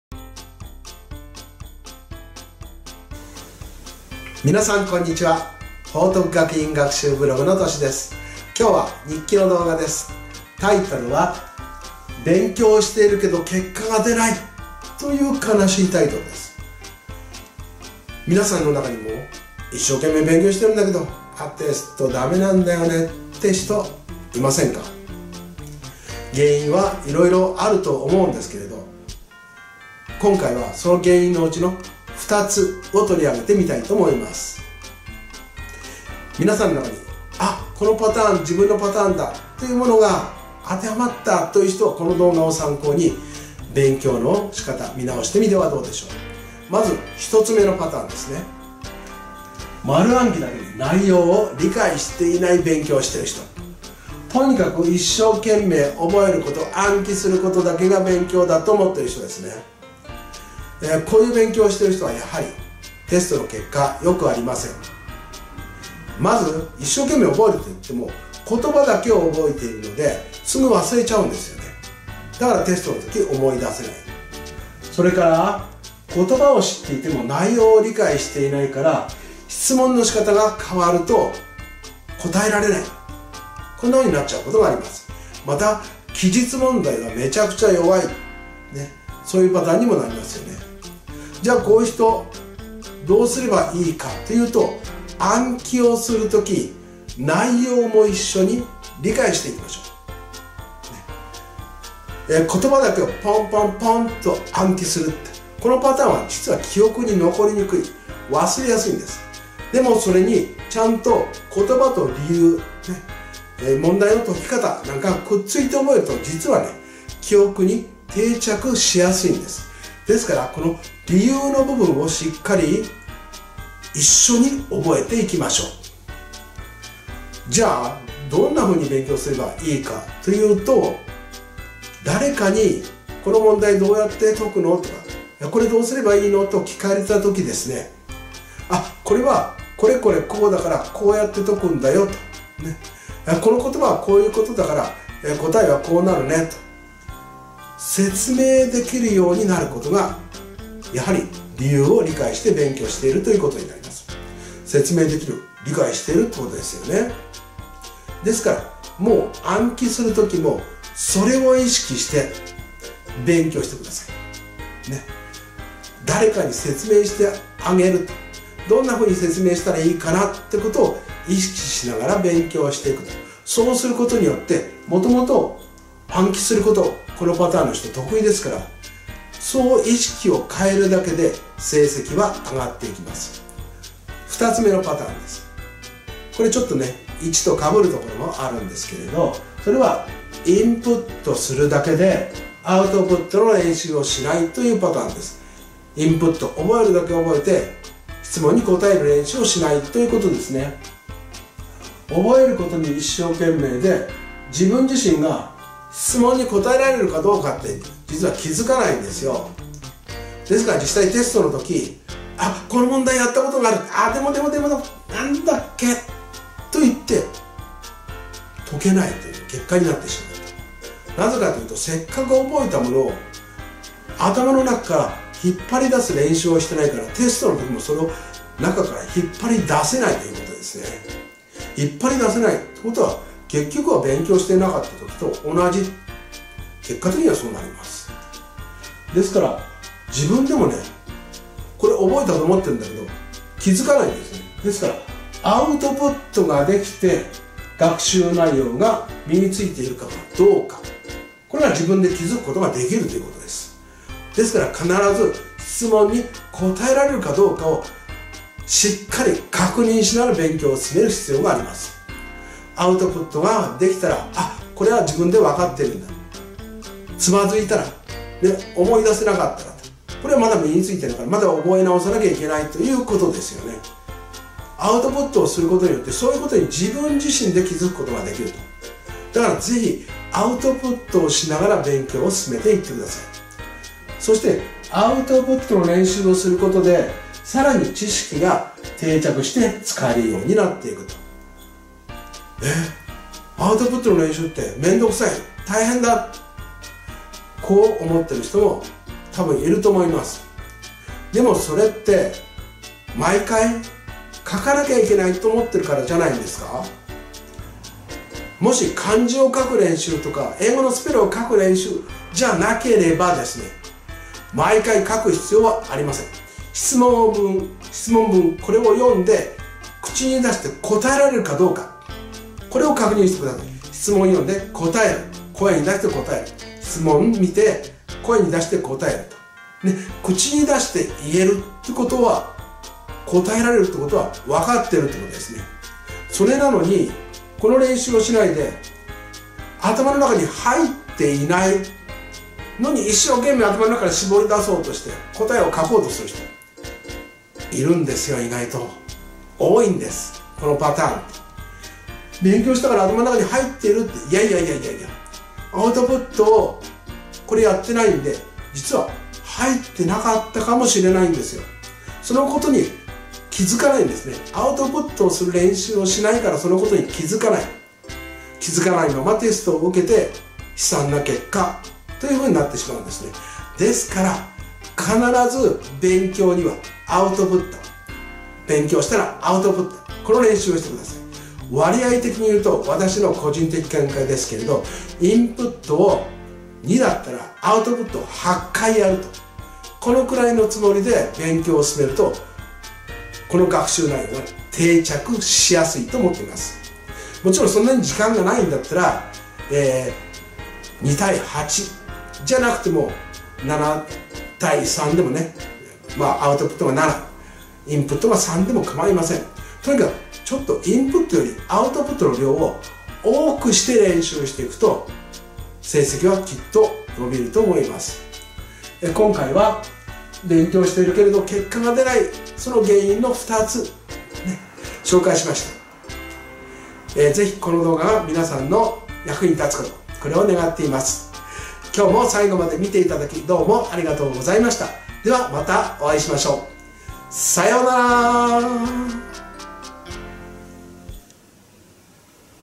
勉強しているけれど結果が出ない 【気ままな日記】
勉強しているけれど結果が出ない原因の2つを取り上げました。